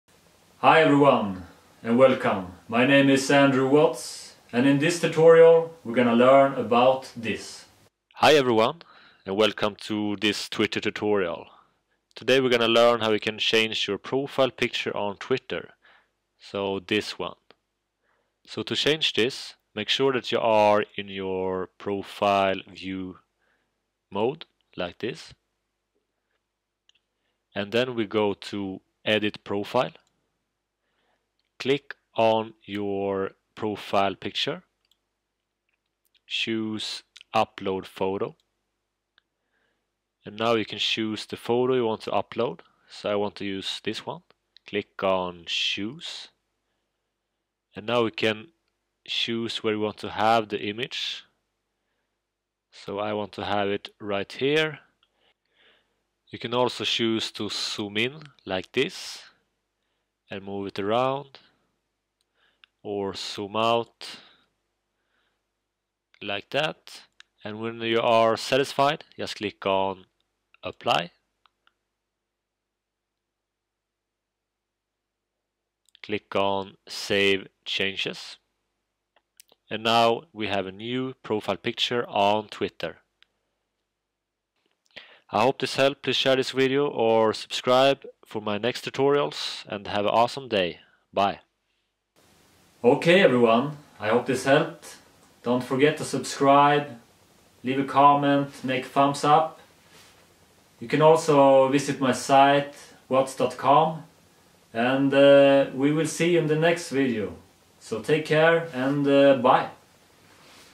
How to change Twitter profile picture / photo - 2016 tutorial
How to easy change your Twitter profile picture with this step by step tutorial in 2016.

Use this video tutorial if you want to change your Twitter photo in minutes.